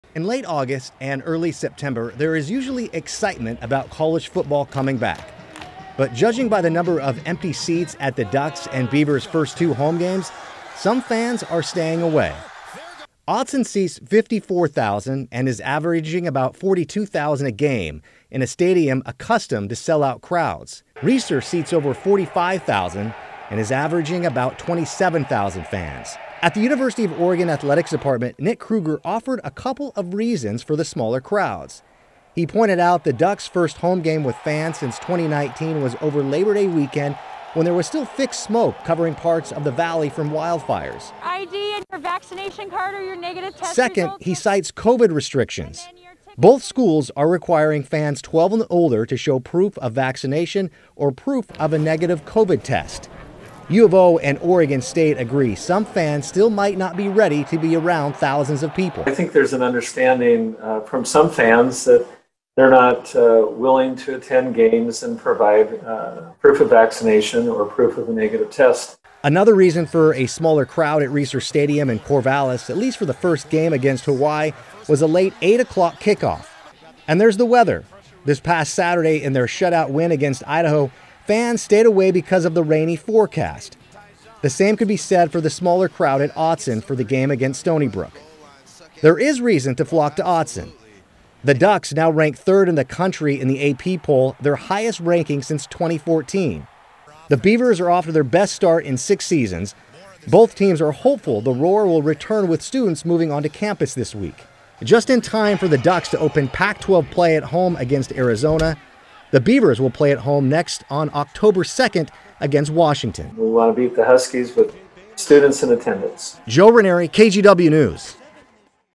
College football games see drop in attendance amid COVID surge, vaccine requirements
College football is  back, but judging by the thousands of empty seats, some fans are reluctant to return to Corvallis and Eugene, and there are a couple of reasons why.

A spokesperson from the University of Oregon says the Duck’s first home game this season was over Labor Day weekend, when thick smoke from wildfires filled the skies over the valley.

Both Oregon and Oregon State University cite COVID restrictions keeping some fans away. The other reason is the fact that students haven’t been on campus yet.

For the school’s next home games, students will be back for classes, and the two schools are hoping that will help bring more people out to catch all of the action.